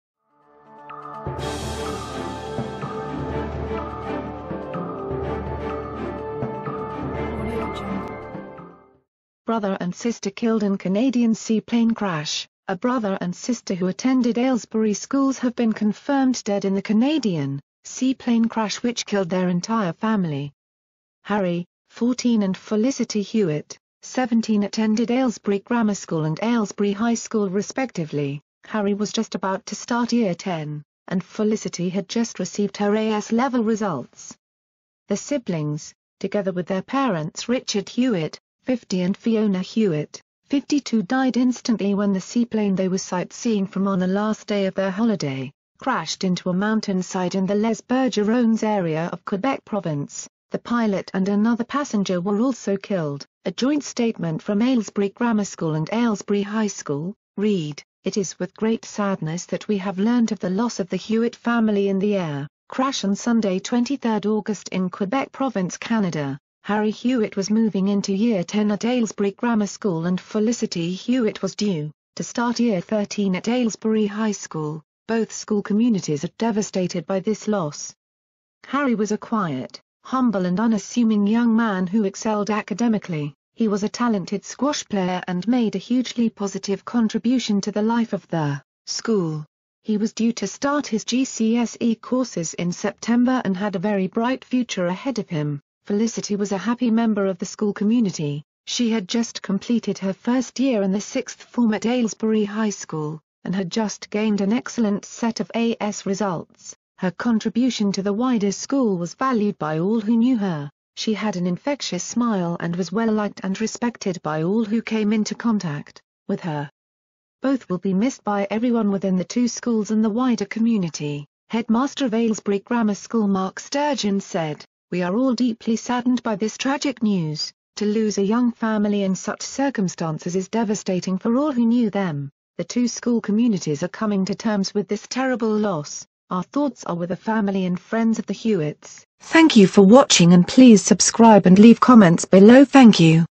Aylesbury news , Aylesbury family die in Canadian seaplane crash
brother and sister killed in Canadian seaplane crash.

A brother and sister who attended Aylesbury schools have been confirmed dead in the Canadian seaplane crash which killed their entire family.

Harry, 14 and Felicity Hewitt, 17 attended Aylesbury Grammar School and Aylesbury High School respectively.
Harry was just about to start year 10, and Felicity had just received her AS level results.

The siblings, together with their parents Richard Hewitt, 50 and Fiona Hewitt, 52 died instantly when the seaplane they were sight seeing from on the last day of their holiday crashed into a mountain side in the Les Bergeronnes area of Quebec province.

The pilot and another passenger were also killed.

A joint statement from Aylesbury Grammar School and Aylesbury High School, read: “It is with great sadness that we have learnt of the loss of the Hewitt family in the air crash on Sunday 23 August in Quebec province Canada.

“Harry Hewitt was moving into year 10 at Aylesbury Grammar School and Felicity Hewitt was due to start year 13 at Aylesbury High School.

“Both school communities are devastated by this loss.

“Harry was a quiet, humble and unassuming young man who excelled academically, he was a talented squash player and made a hugely positive contribution to the life of the school. He was due to start his GCSE courses in September and had a very bright future ahead of him.

“Felicity was a happy member of the school community. She had just completed her first year in the sixth form at Aylesbury High School, and had just gained an excellent set of AS results.

“Her contribution to the wider school was valued by all who knew her. She had an infectious smile and was well liked and respected by all who came into contact with her.

“Both will be missed by everyone within the two schools and the wider community.”

Headmaster of Aylesbury Grammar School Mark Sturgeon said: “We are all deeply saddened by this tragic news. To lose a young family in such circumstances is devastating for all who knew them.

“The two school communities are coming to terms with this terrible loss. Our thoughts are with the family and friends of the Hewitts.”